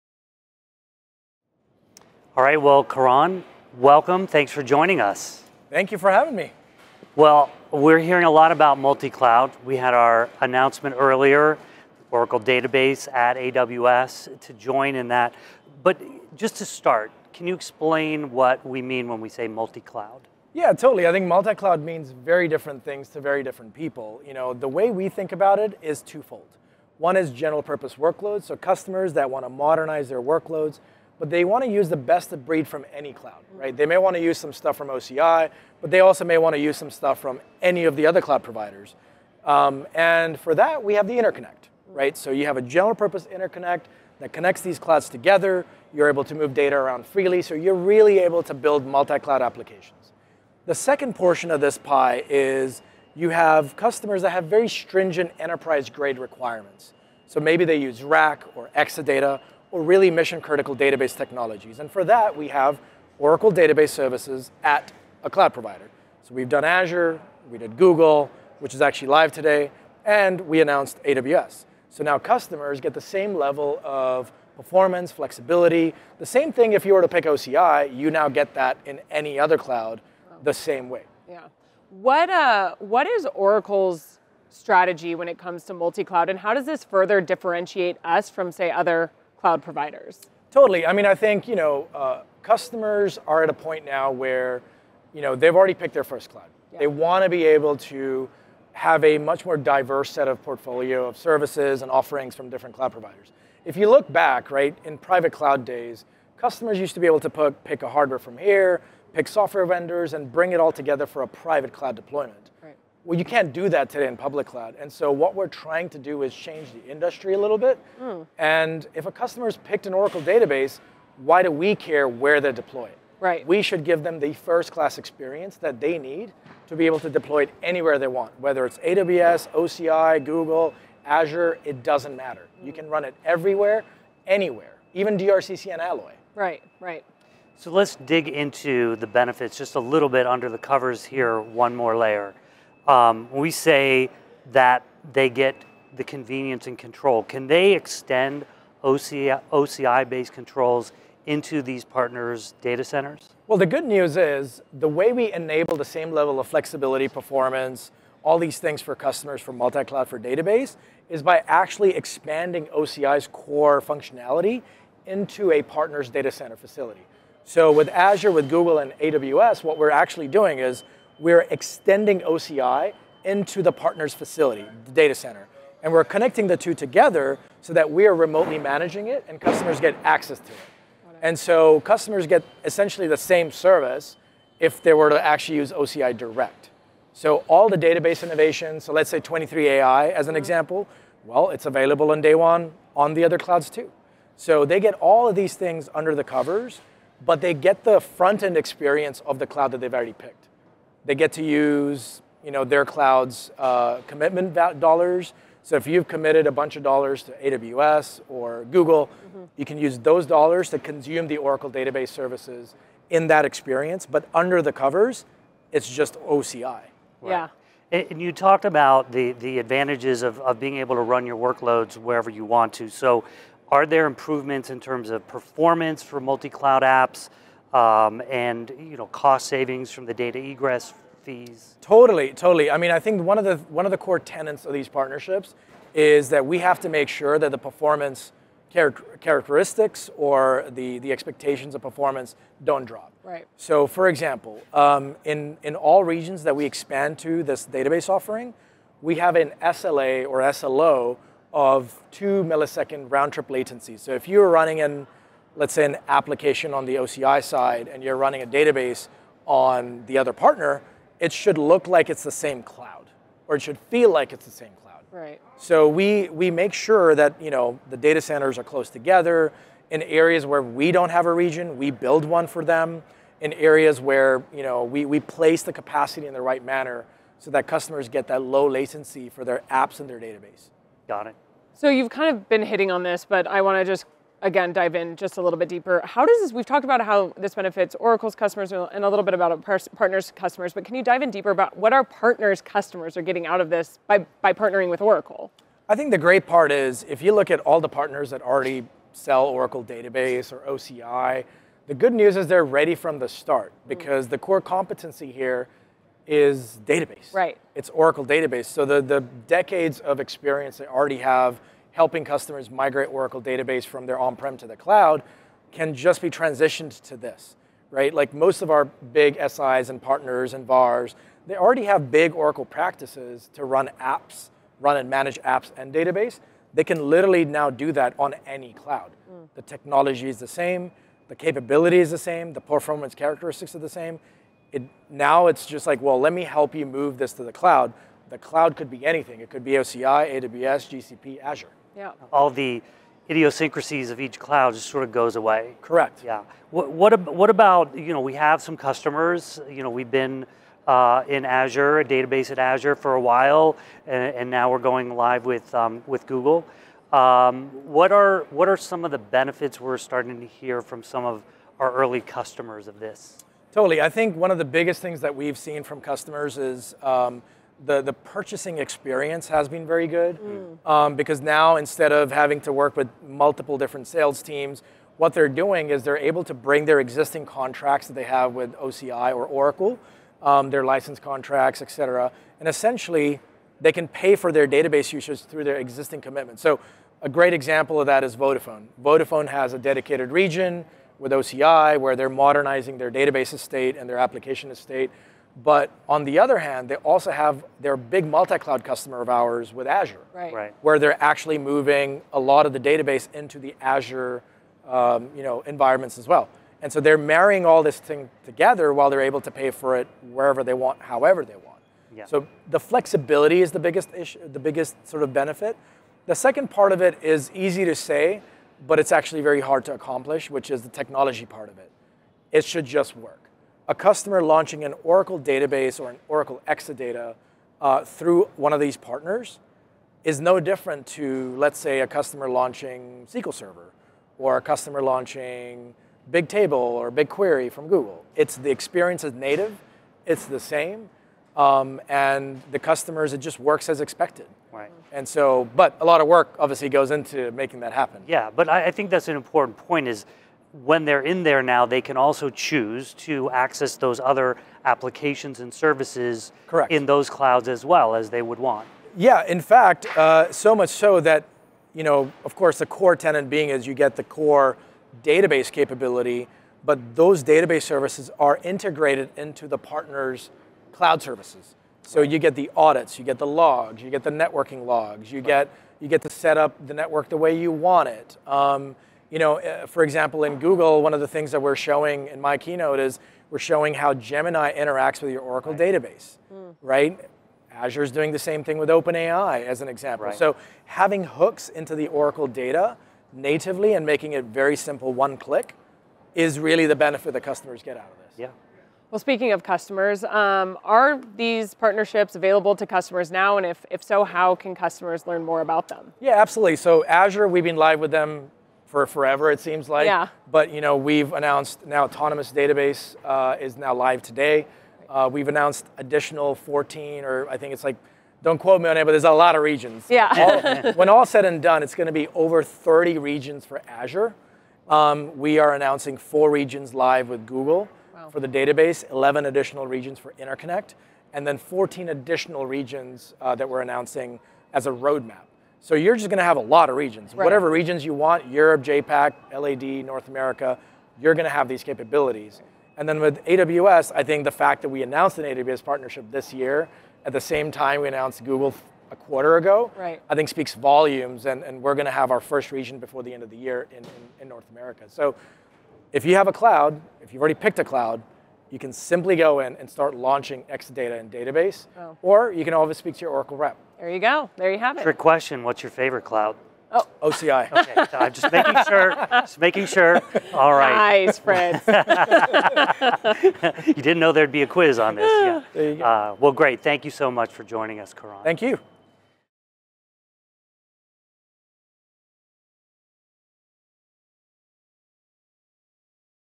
Oracle TV CloudWorld 2024: Oracle's Approach to Multicloud
Karan Batta, Senior Vice President, Oracle Cloud Infrastructure, joins Oracle TV to discuss Oracle's multicloud strategy and how Oracle customers are benefitting from our latest partnership announcements.